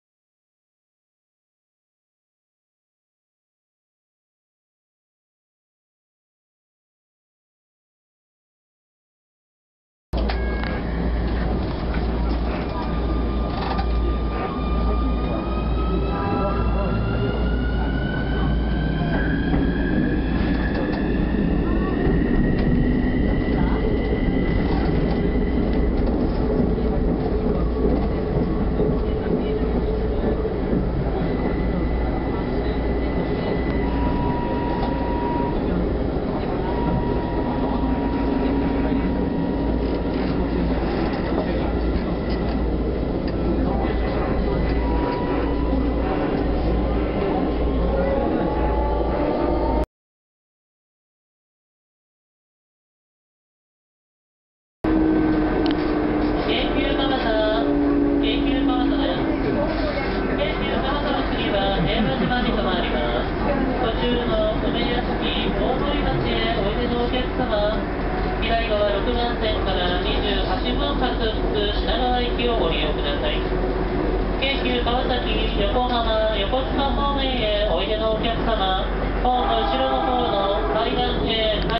第1京浜国道高架橋の曲線線路部の噴霧装置.wmv
京急空港線高架化の線路摩耗緩和装置運用
京急蒲田駅第1京浜国道高架橋上の曲線レール部の摩耗緩和噴霧装置
急カーブ線路の摩耗を防止するため
2010年10月29日誤記訂正